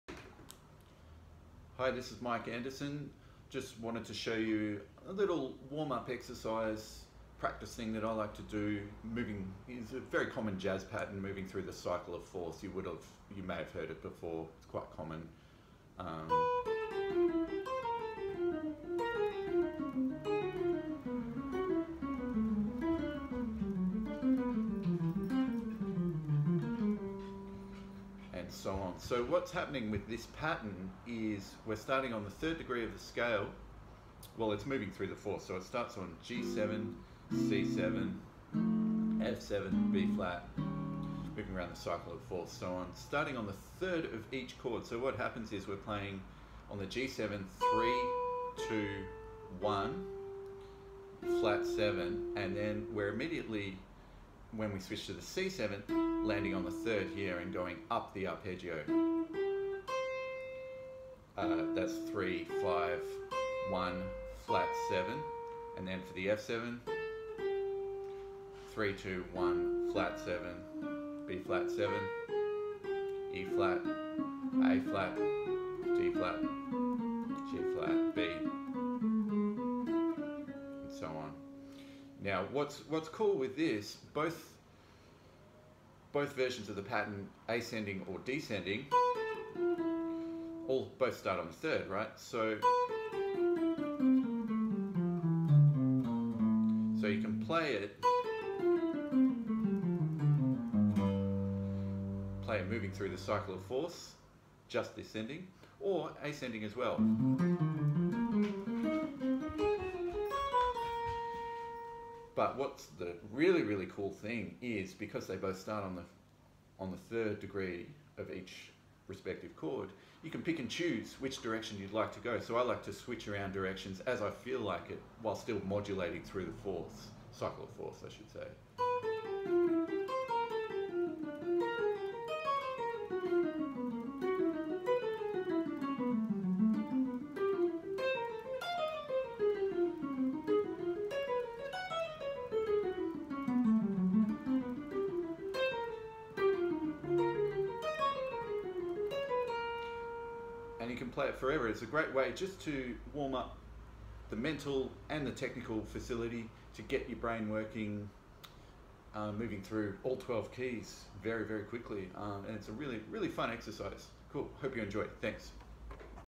Workout Wednesday w/ Mike Anderson
Howdy!
Here's a little WorkoutWednesday with Ensemble & Guitar Teacher, Mike Anderson 🎸
Get your gat chops up by moving around the Circles of Fourths like so! 🔵